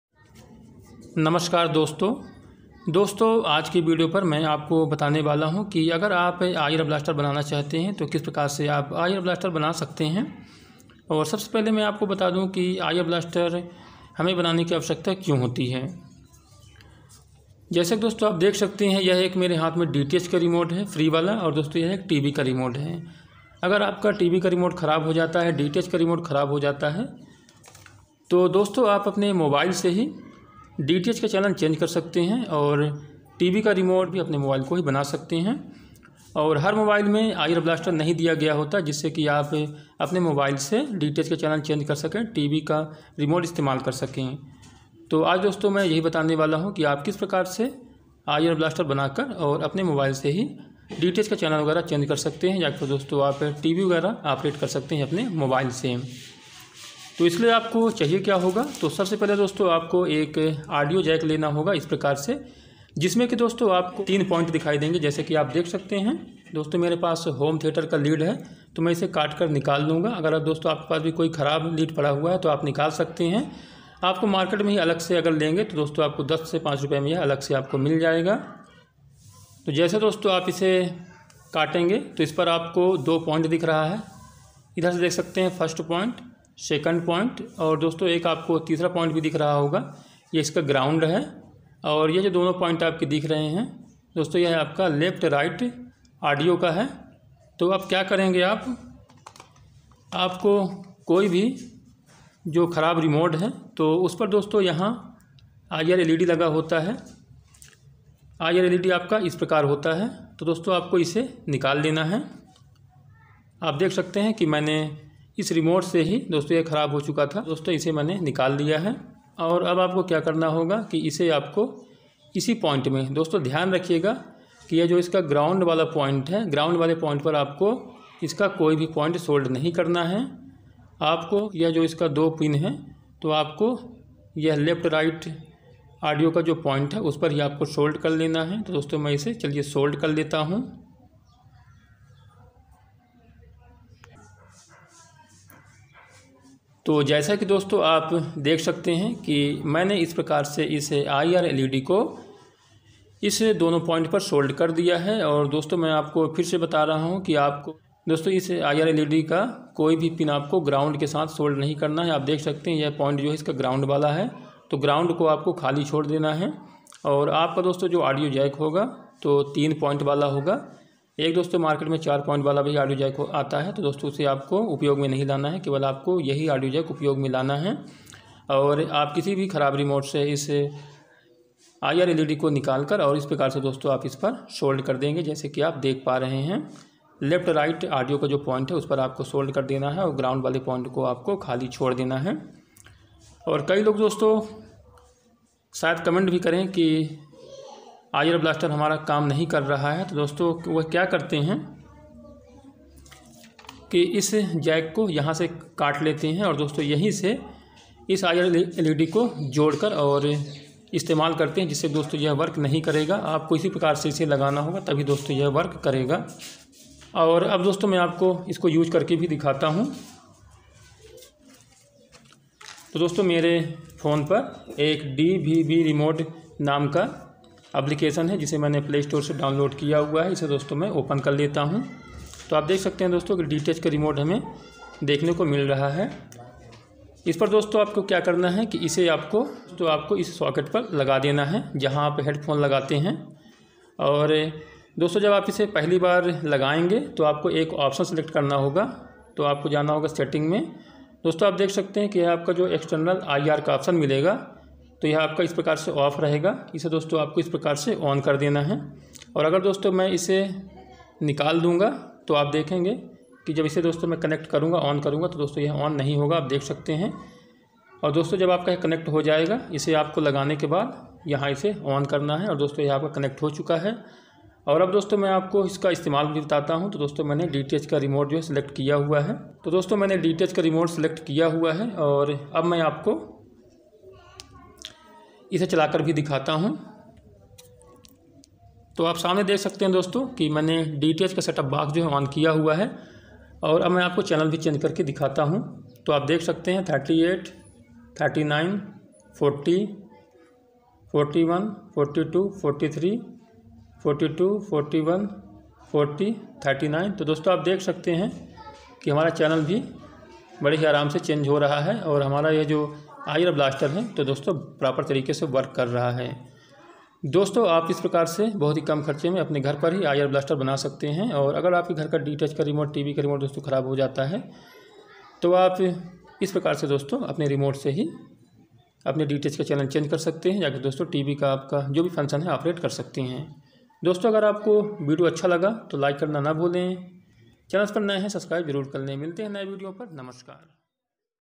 How to make IR Blaster at home 15 Rupees Only 100% working | Mobile 📱 phone को रिमोट कैसे बनाएं
How to make IR blaster simple trick.